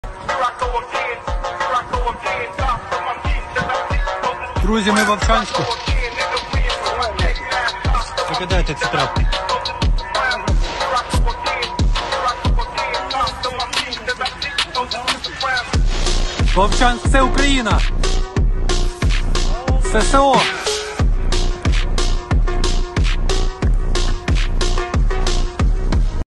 Volchansk at home.
Volchansk at home. The border guards showed how they restore order in the city liberated from Russian invaders The city has been under occupation by Russian troops since February 24, 2022. Russian rags are burning, propaganda posters are torn, "Glory to Ukraine!" - sounds in every liberated locality! Everything is as it should be!